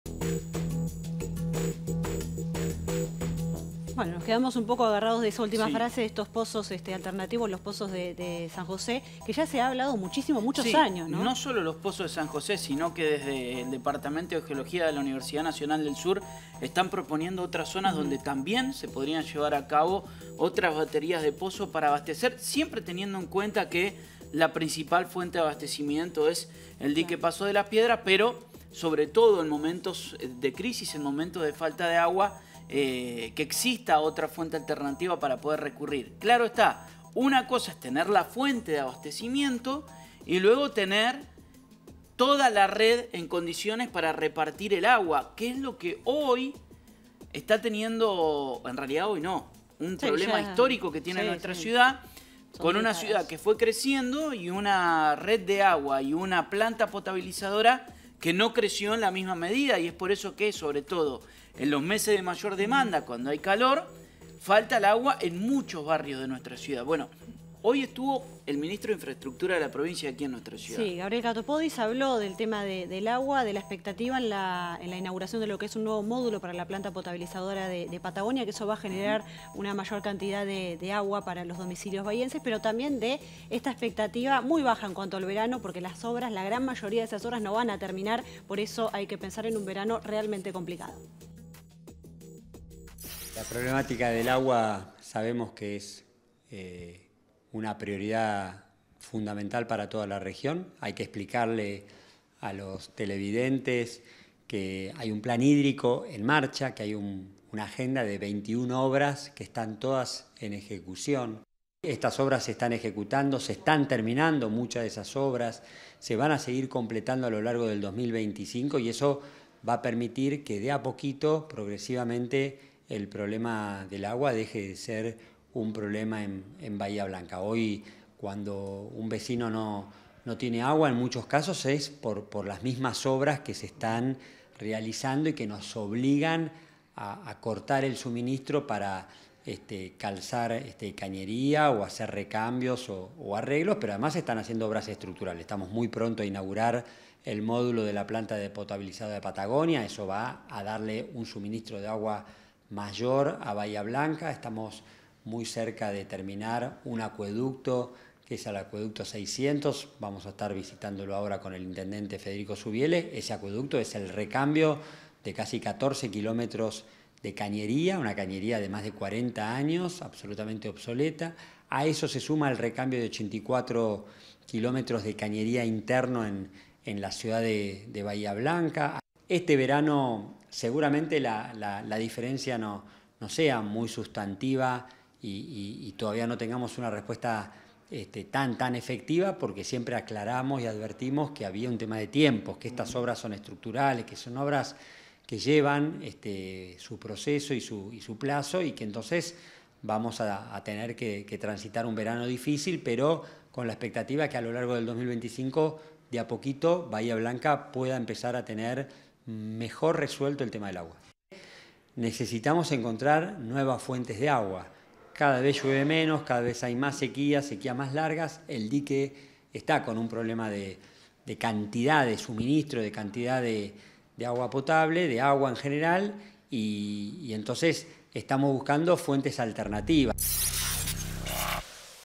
La crisis del agua: se viene otro verano difícil
Si bien se están llevando a cabo las obras necesarias para mejorar el abastecimiento de agua en nuestra ciudad, el Ministro de Infraestructura bonaerense, Gabriel Katopodis, anticipó que no llegarán a tiempo para los meses de mayor calor.